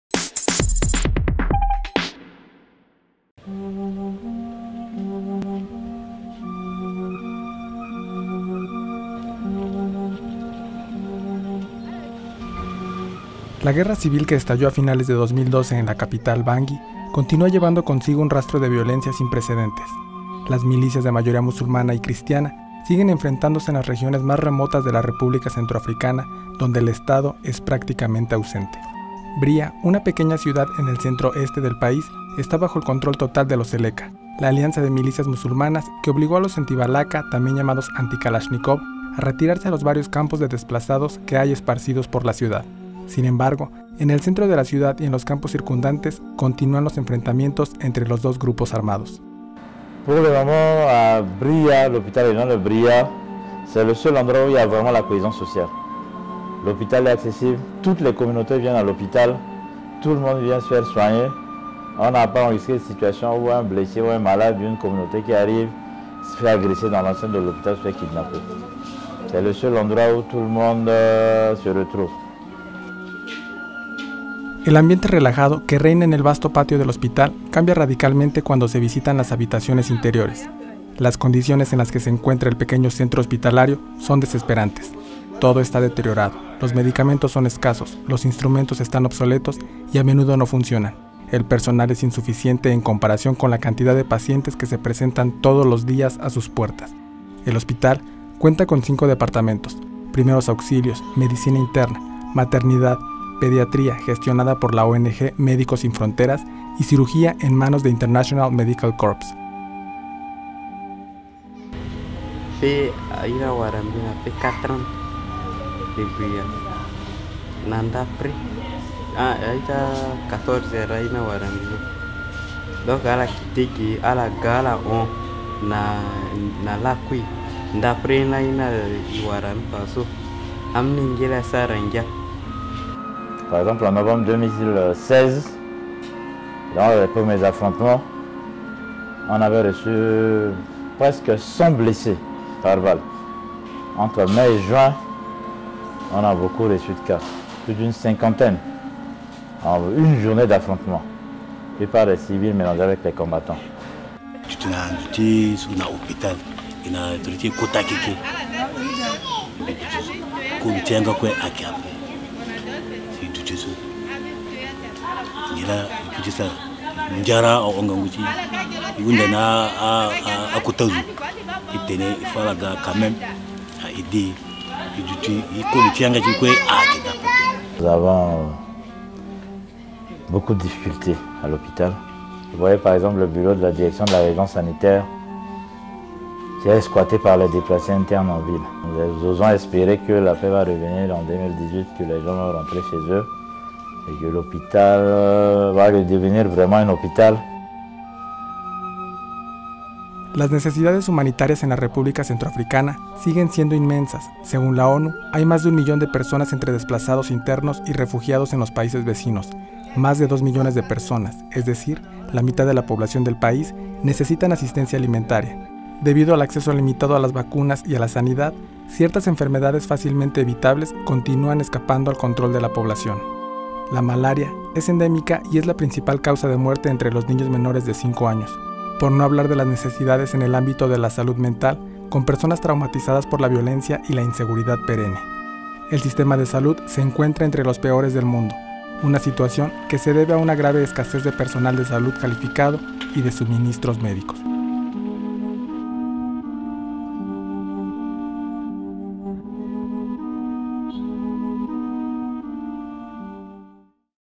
Hospital Regional Universitario de Bria
El Hospital Regional Universitario de Bria, pesar de sus deficiencias en términos de higiene y salud sigue siendo el único lugar de cohesión social de la región